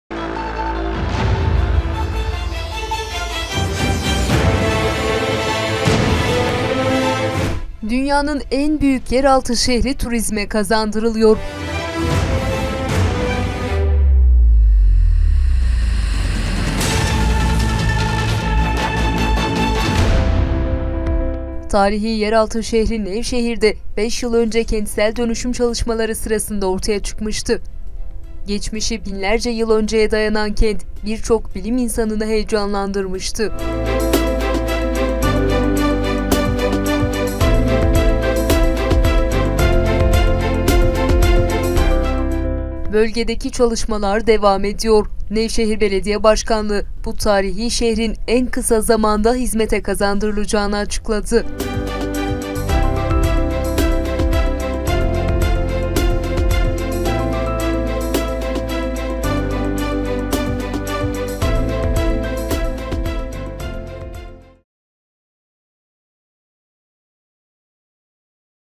DÜNYANIN EN BÜYÜK YERALTI ŞEHRİ TURİZME KAZANDIRIILIYOR
Nevşehir’de yaklaşık 5 yıl önce başlayan Nevşehir Kalesi ve Çevresi Kentsel Dönüşüm Projesi sırasında dünyanın en büyük yeraltı yerleşimi keşfedilmişti. Nevşehir Belediye Başkanı Rasim Arı dünyanın en büyük yeraltı şehrini en kısa zamanda hizmete kazandırılacağını açıkladı.
Nevşehir Belediye Başkanı Rasim Arı, Nevşehir Belediyesi tarafından projelendirilen ve TOKİ ile işbirliği içerisinde yürüttükleri ‘Nevşehir Kalesi ve Çevresi Kentsel Dönüşüm Projesi’ çalışmaları sırasında ortaya çıkartılan dünyanın en büyük yeraltı şehrini en kısa zamanda hizmete kazandırılacağı müjdesini verdi. Belediye Başkanı Rasim Arı, “Seçim döneminde vaat ettiğimiz ne varsa bu vaat ettiklerimizin tamamının arkasındayız. O gün fiziksel ve zihinsel değişim ile ilgili ifade ettiğimiz her şeyi uygulamaya çalışıyoruz. Birincisi vatandaşlarımızla, hemşerilerimiz ile birlikte olacağımızı ifade etmiştik sizde şahitlik ediyorsunuz ki bir belediye başkanı sıfatı ile değil bir ağabey, bir kardeş ve bir evlat sıfatıyla kucaklaşıyoruz. Bununla birlikte bu şehri marka şehir haline getirecek bu şehri dünyada ve Türkiye’de tanınır bir şehir haline getireceğimizi bunun için fiziksel değişim yapacağımızı söylemiştik. Onlardan bir tanesi de kale bölgesinin turizme açılmasıydı. Geldiğim ilk günden beri en fazla üzerinde mesai harcadığım konu kale bölgesinin turizme açılmasıdır. Kale bölgesinde var olan sorunları çözmeye başladık. Çünkü orada biliyorsunuz farklı insanlarımızın konutları ve hakları var. O hakları hak sahiplerinden alarak bu sorunu ortadan kaldırmaya yani mülkiyet sorununu ortadan kaldırmaya çalışmıştık. Bu sorunlarımız aşağı yukarı bitti. Geçen hafta İller Bankası ekibiyle ve akabinde yine Ankara’dan gelen bir ekiple bu konuları istişare etmiştik. Bugün Çinliler ile bir araya geldik. İnşallah Çin’den bir eyalet başkanı ile bir araya geleceğiz aynı şekilde devam ettiriyoruz. Çok yakın zamanda etütleri yapıyoruz, projelerini çıkartacağız. İnşallah bu şehrin hak ettiği değeri kazandıracak ve bu şehrin kaderini değiştirecek kale bölgesini ve etrafındaki dünyanın en büyük yeraltı şehrini şehrimizin menfaatlerine sunacağız. Çalışmalar son sürat devam ediyor, en kısa zamanda burayı gerçekleştireceğiz” diye konuştu.
(İHA)